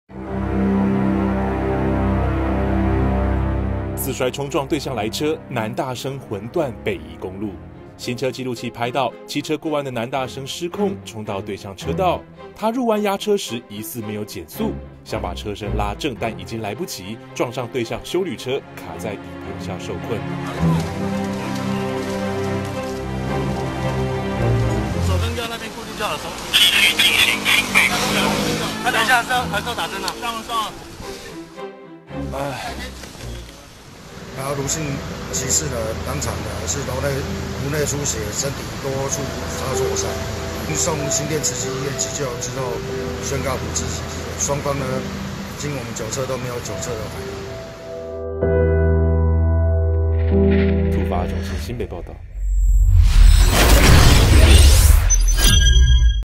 男大生魂斷北宜 行車紀錄器還原現場 | 台灣蘋果日報
新北市新店北宜公路，昨天下午4時許發生一起死亡車禍，1名19歲盧姓男大生騎機車失控衝入對向車道，與休旅車發生撞擊後，人卡入車底導致頭部重創送醫不治，警方事後檢視休旅車行車紀錄器，發現事故原因可能是盧男過彎時速度過快，導致車輛失控打滑。